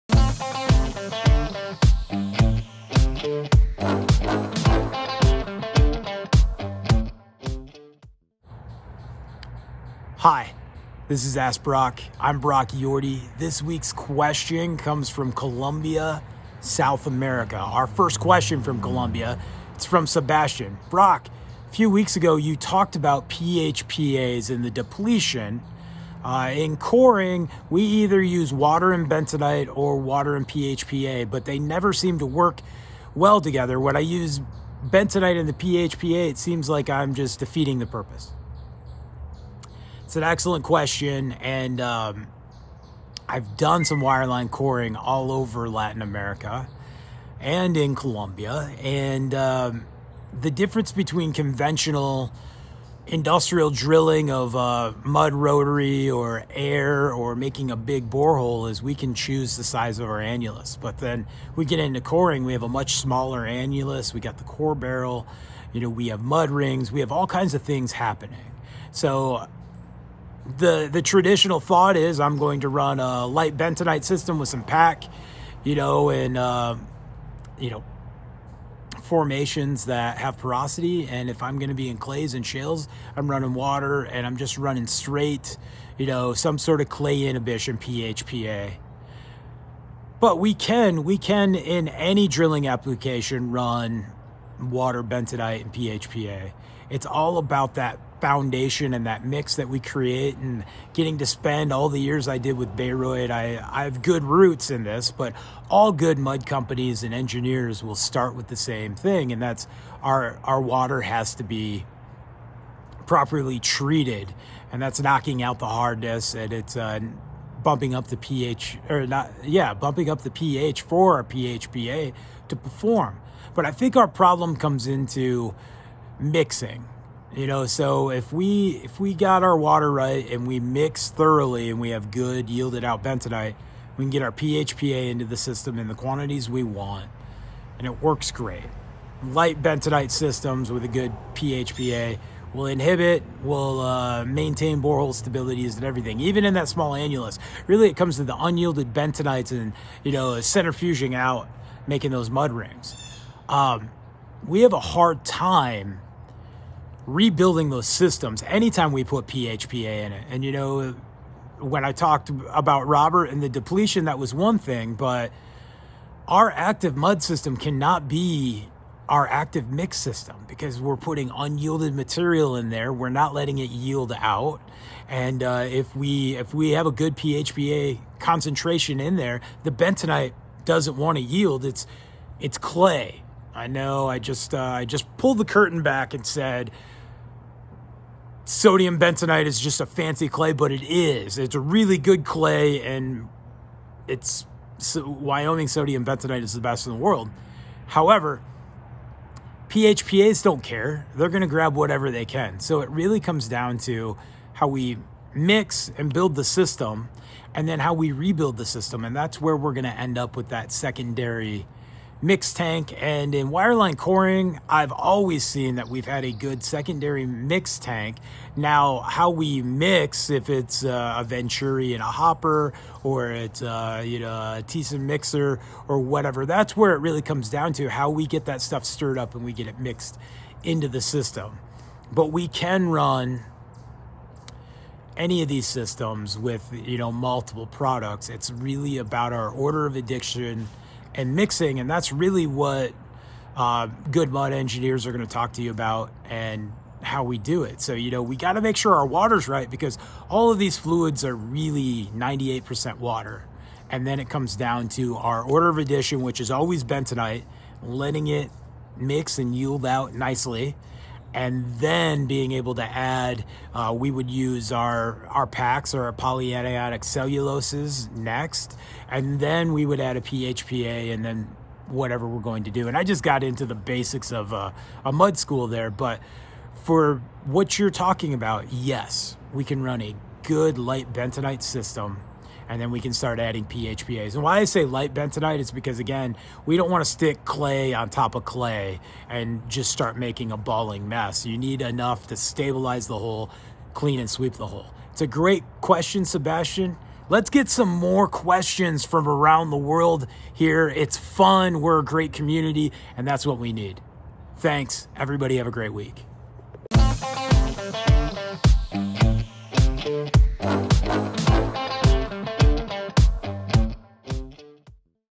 Tips for Running Bentonite, PHPAs Together on Drilling Projects
In Ask Brock episode 133, host Brock Yordy talks water and proper yield before adding PACs, PHPAs.